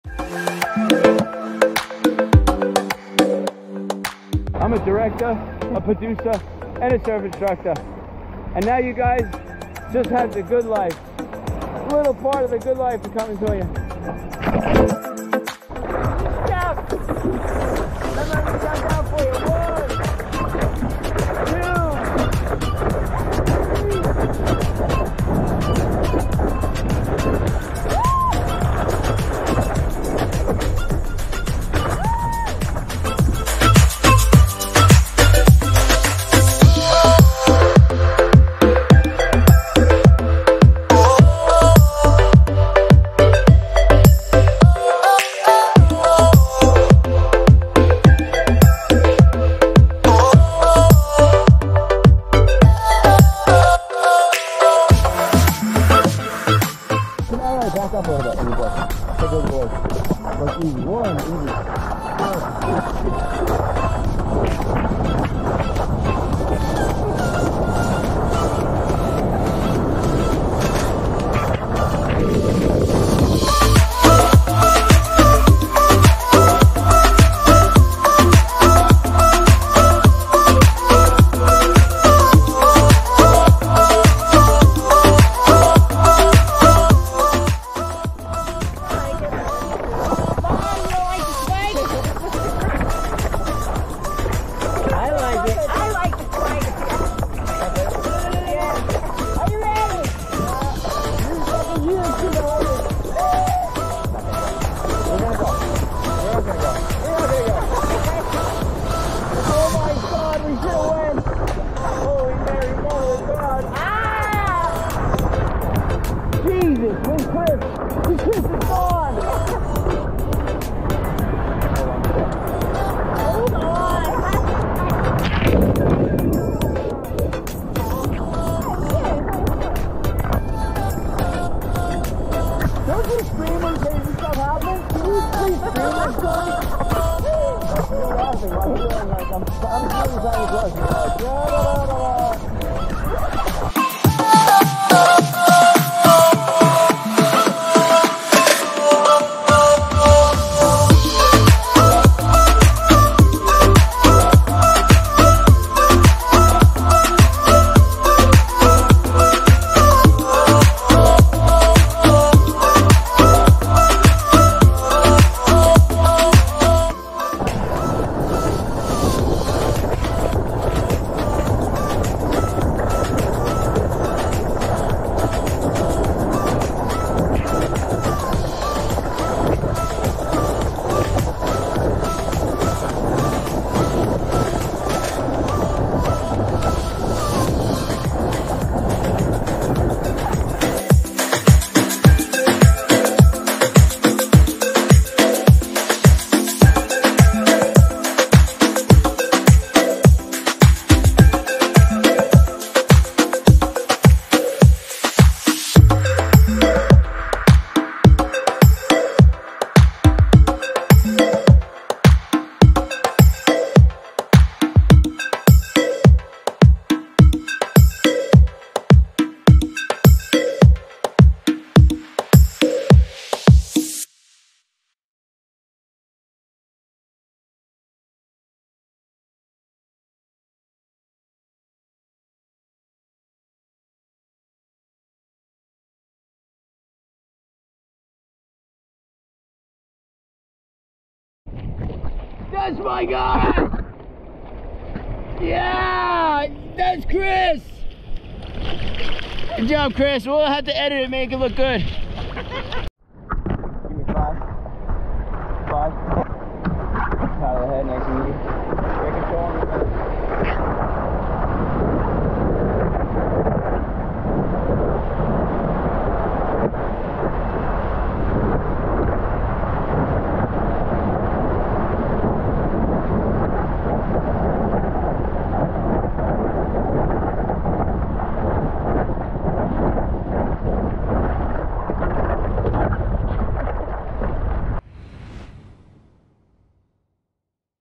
Chris & Stephanie 2022 back for the holidays
Brother & sis surf lesson on the island of Oahu with islandFielSurf.com